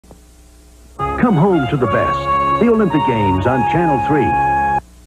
WSTM (NBC) Station ID 1988
This is WSTM's station ID from the Olympics in Seoul in 1988
WSTM is located in Syracuse, NY